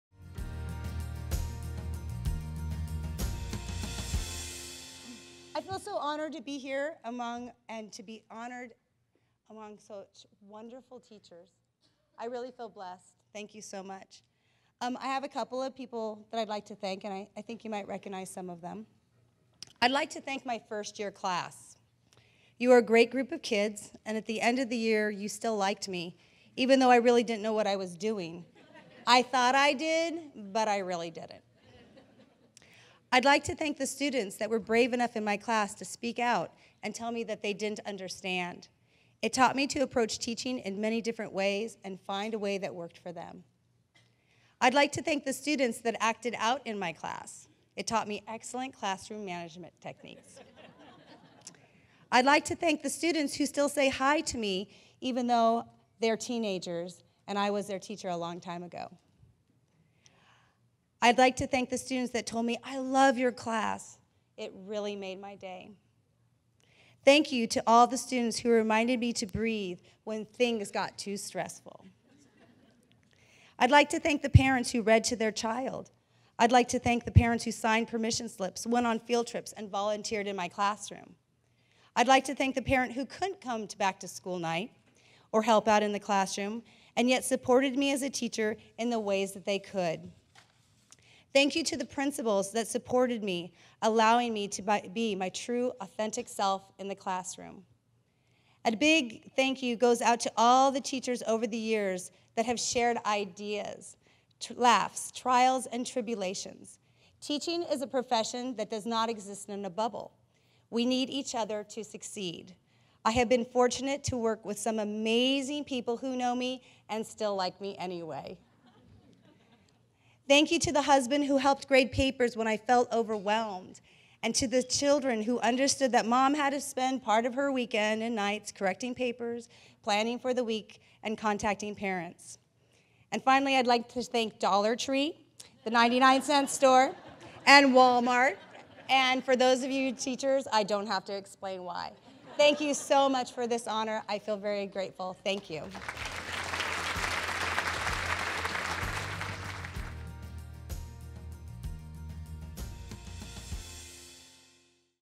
Kendall Murphy: Sacramento County Teachers of the Year 2017 Awards Speech
The Sacramento County Teachers of the Year program recognizes excellence in teaching within the county's schools. Participating districts nominate individual teachers to receive this special recognition. This speech was delivered at the Teachers of the Year awards recognition dinner where two teachers were identified as the Sacramento County Teachers of the Year and became eligible to participate in the statewide competition.

The comments made in this video don't necessarily represent SCOE's positions, strategies, or opinions. Video courtesy secctv.org; published with permission.